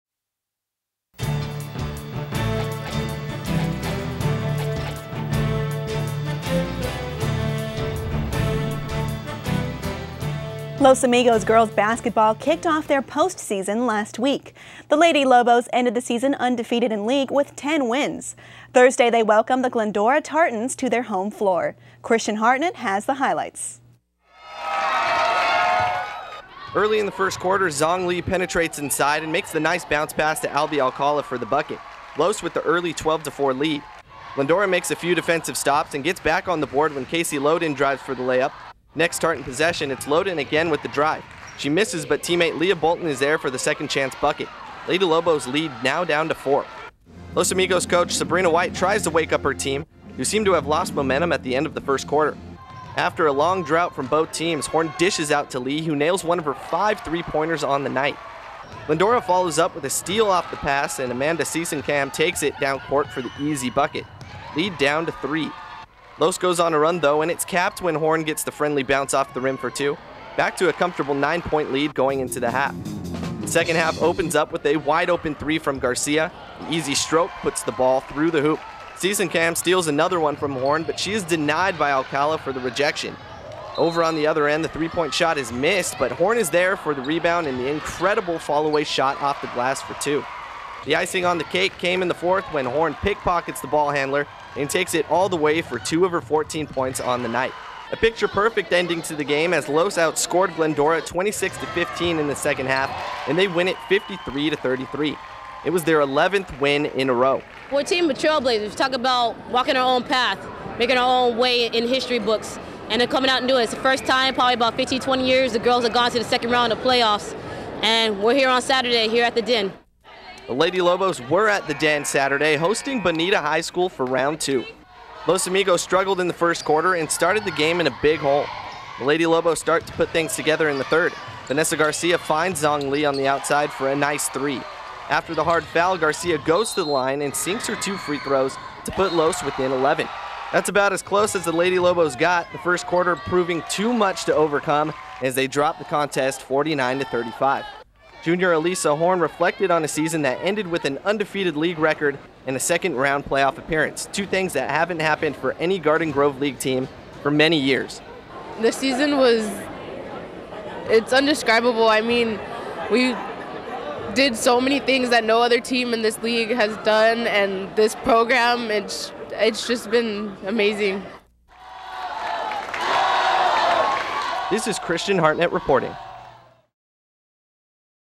Lady Lobos Playoff Hoops Highlights
Los Amigos girls basketball kicked off their post-season last week. The Lady Lobos ended the season undefeated in League with 10 wins. Thursday they welcomed the Glendora Tartans to their home floor. Garden Grove TV3 reporter Christian Hartnett has the highlights.